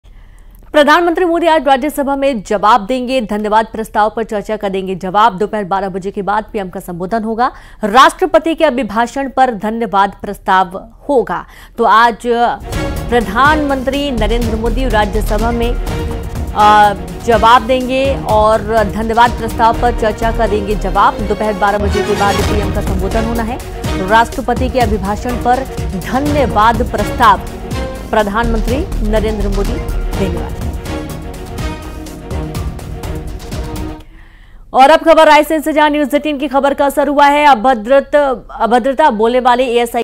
Parliament Session 2024 : PM Modi आज राज्यसभा में विपक्ष के आरोपों का दे सकते हैं जवाब | Latest News
PM Modi आज राज्यसभा में विपक्ष के आरोपों का दे सकते हैं जवाब. धन्यवाद प्रस्ताव पर चर्चा का जवाब देंगे PM Modi. दोपहर 12 बजे के बाद PM Modi का संबोधन होगा.

n18oc_elections
About: